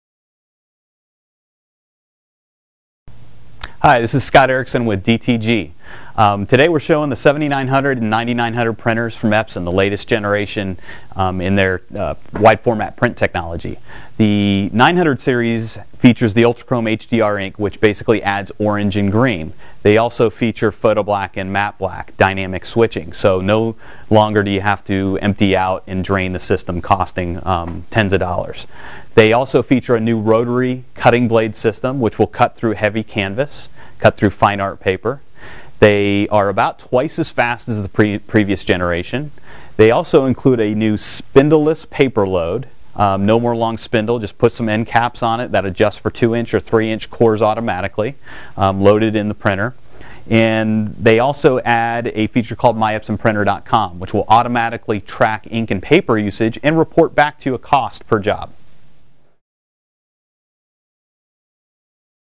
A short look at the Epson 7900 and 9900 printers
- Video shows you what is new with the epson 7900 and 9900 printers.